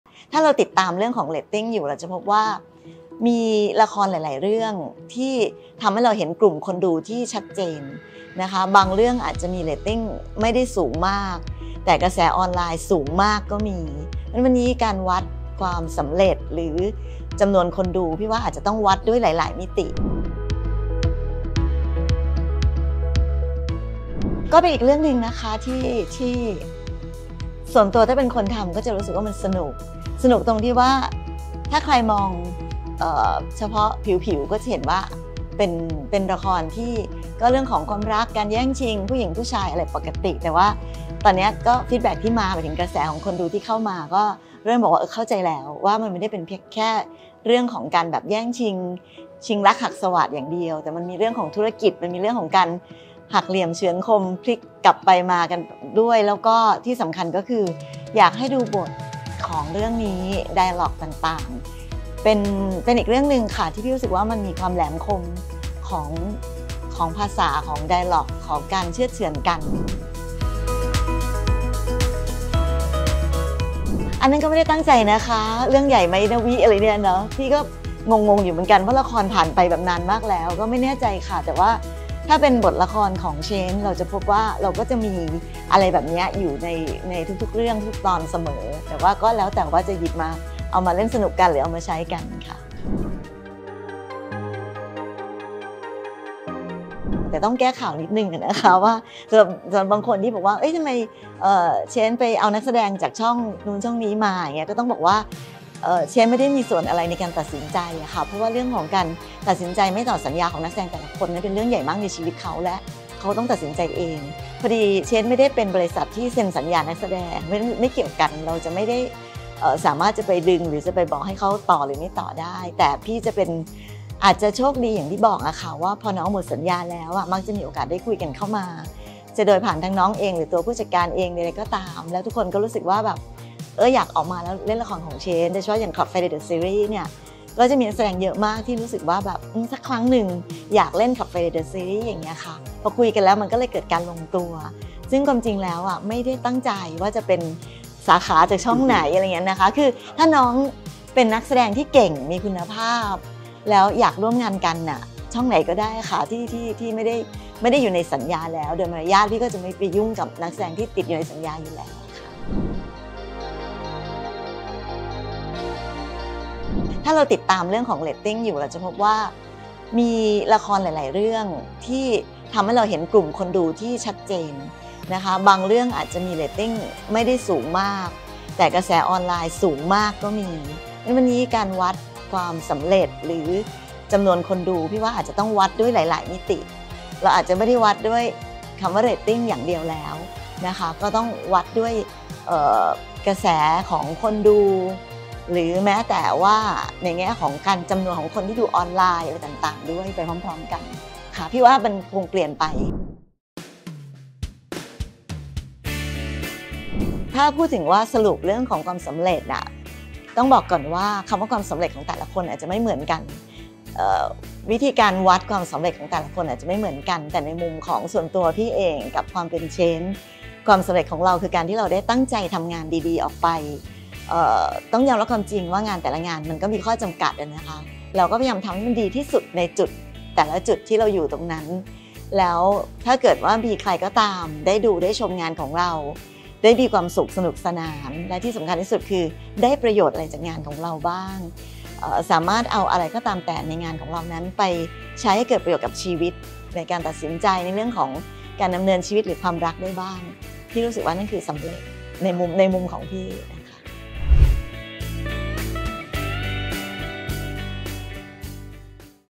สงครามเรตติ้ง ที่ไม่ได้มีแค่ทีวีอีกต่อไป | EP. 2 | NewsClear_ประเด็นร้อน
ในยุคที่เทคโนโลนีกำลังพัฒนาไปอย่างก้าวกระโดด จนทำให้บริบทหลายๆอย่างในสังคมเปลี่ยนไป หนึ่งในนั้น คือการวัดเรตติ้ง ที่ไม่ใช่แค่ในทีวี แต่ยังหมายถึงสื่ออื่นๆบนโลกออนไลน์อีกด้วย ไปดูมุมมองในเรื่องนี้กับ พี่ฉอด สายทิพย์ มนตรีกุล ณ อยุธยา กับคำว่า “สงครามเรตติ้ง” รวมไปถึงสูตรความสำเร็จของ “Change 2561”